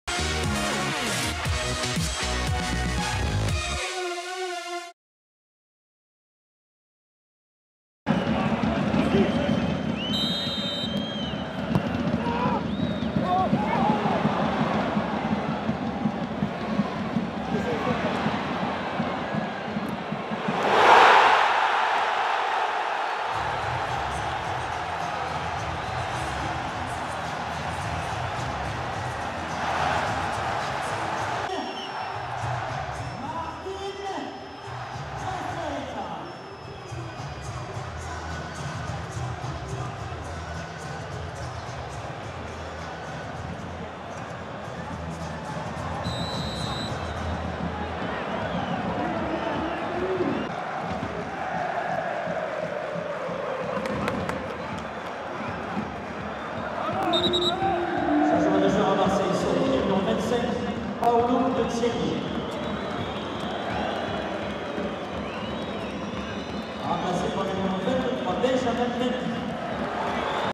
But Martin BRAITHWAITE (67') / Toulouse FC - Olympique de Marseille (1-1) - (TFC - OM) / 2015-16
Toulouse FC - Olympique de Marseille (1-1) - But de Martin BRAITHWAITE (67'). Tous les buts de Toulouse FC - Olympique de Marseille en vidéo.

Stadium - mercredi 23 septembre 2015

Buteurs : Martin BRAITHWAITE (67' - Toulouse FC) / Michy BATSHUAYI (90' - Olympique de Marseille)